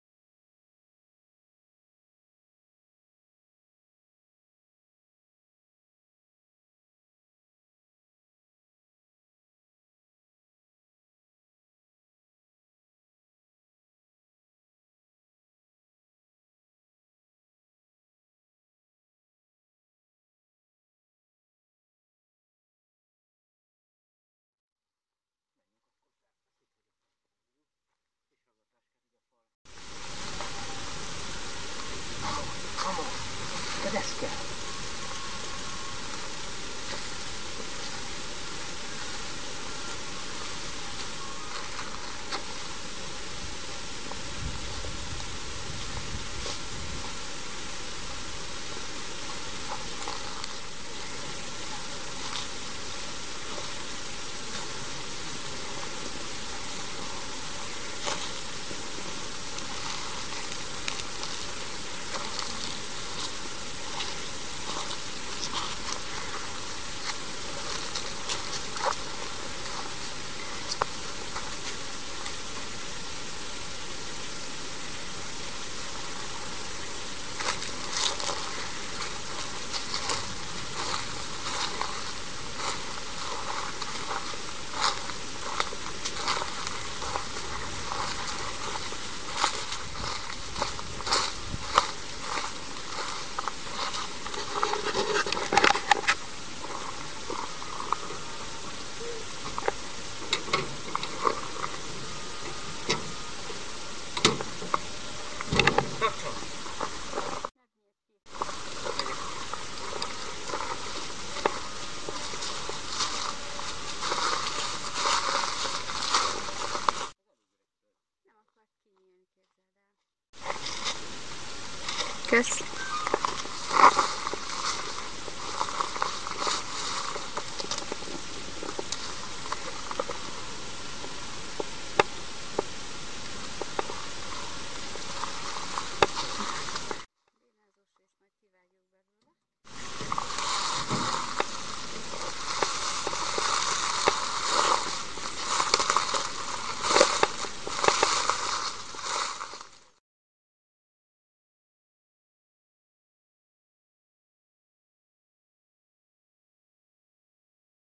Bükkalja - Kács 2. 2015.08.15
A kácsi Kálvária-domb bemutatása.